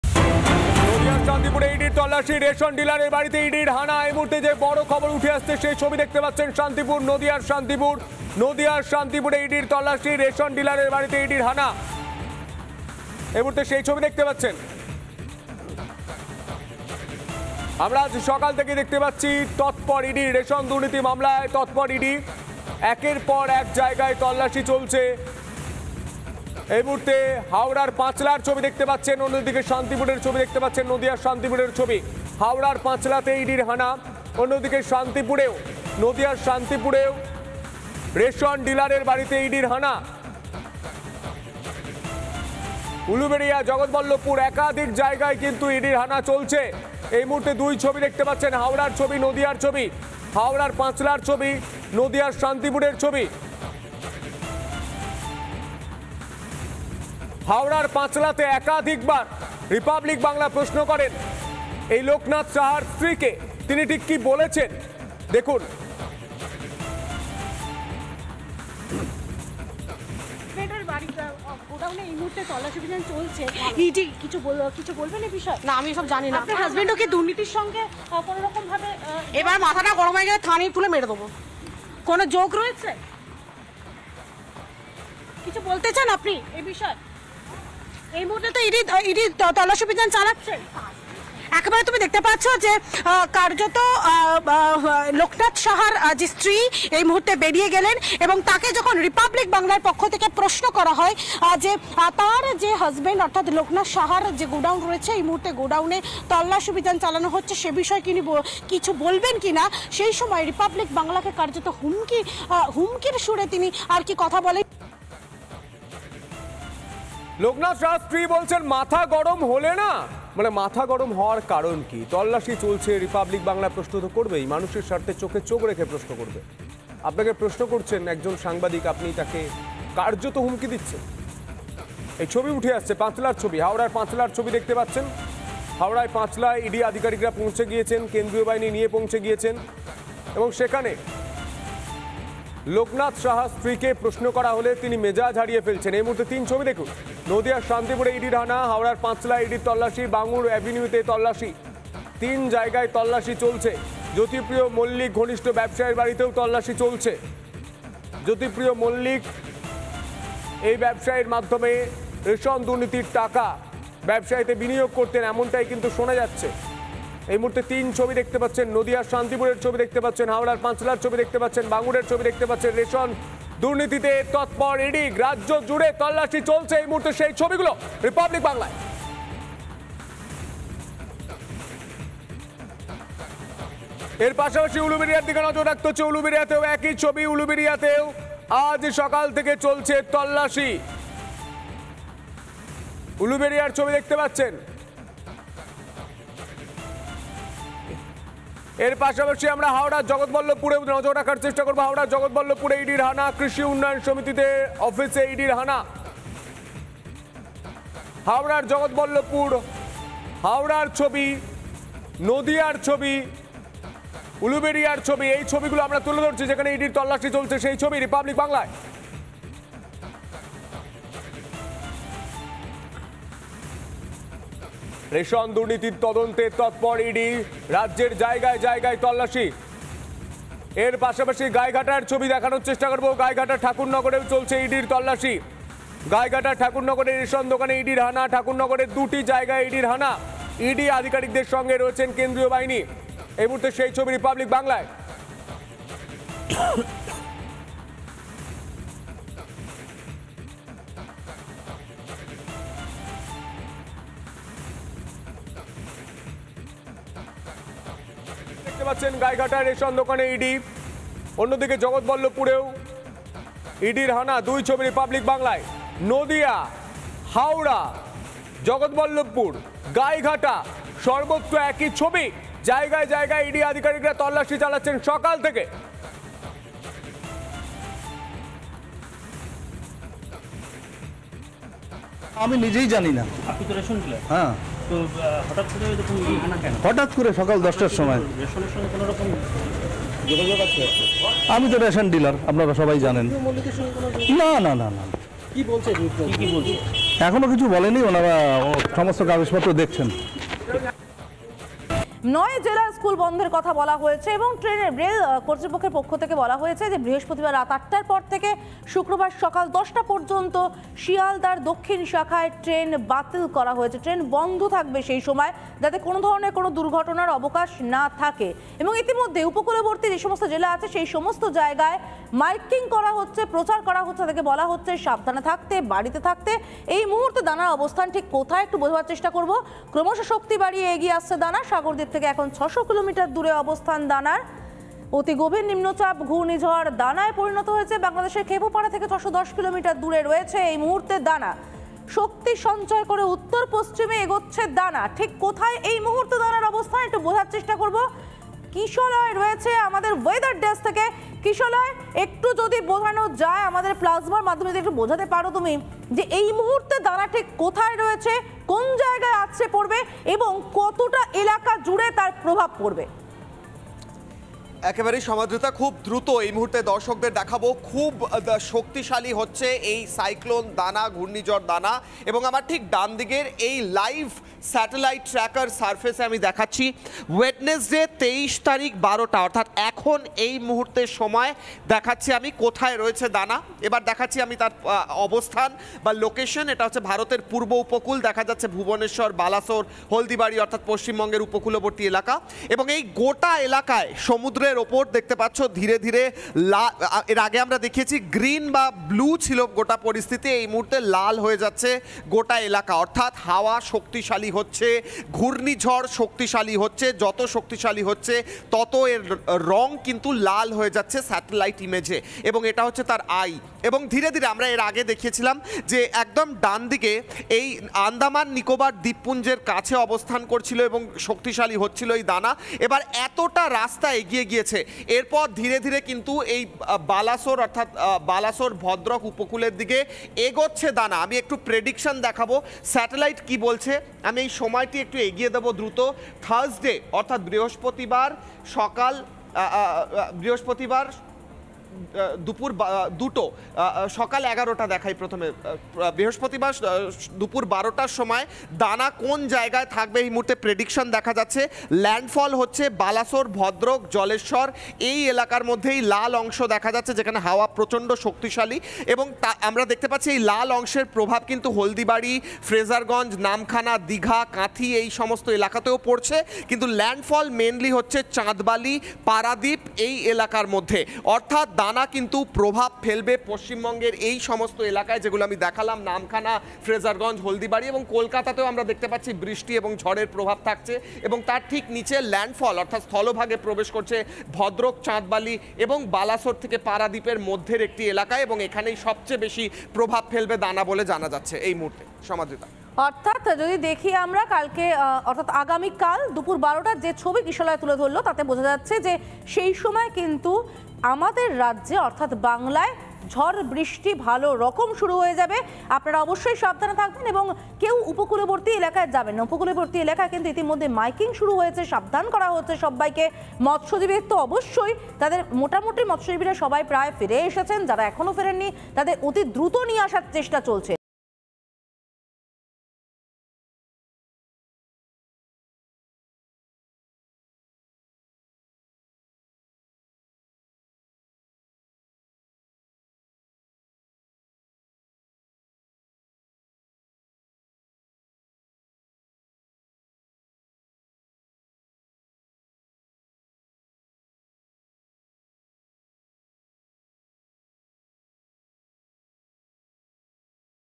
নদিয়ার শান্তিপুরে রেশন ডিলারের বাড়িতে ইডির হানা। হাওড়ার পাঁচলাতেও ইডির তল্লাশি
নদিয়ার শান্তিপুরে রেশন ডিলারের বাড়িতে ইডির হানা। হাওড়ার পাঁচলাতেও ইডির তল্লাশি। উলুবেড়িয়া, গাইঘাটা, জগৎবল্লভপুরে ইডির তল্লাশি।

রিপাবলিক স্বাধীন।
রিপাবলিক নির্ভীক।
আপনিই রিপাবলিক।